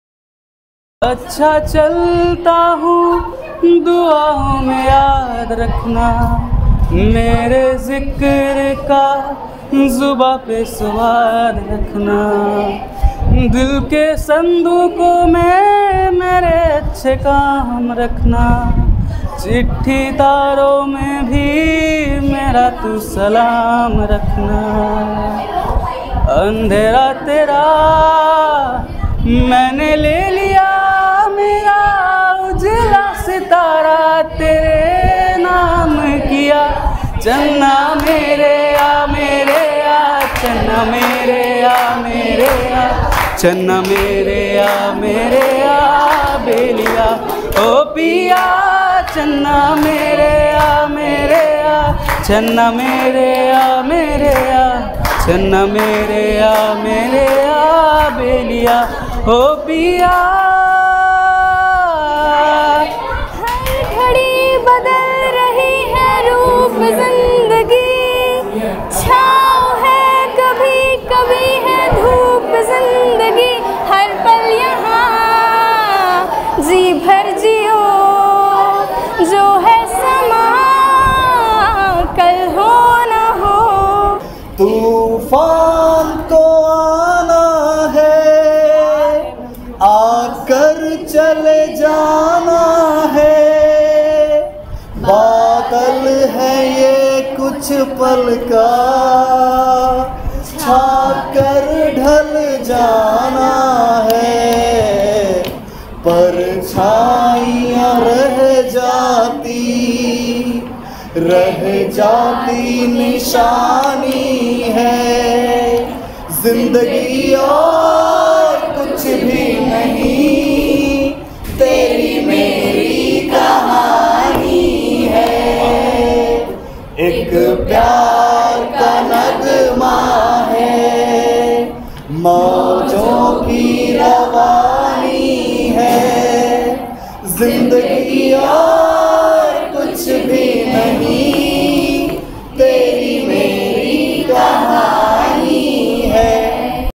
Teachers' Day Celebration with My Lovely Students ll Teachers' Day 2024 ll
Hello friends,
This is a worth cherishable moment of Teachers' Day Celebration with My Lovely class 12 Students of 2024 batch .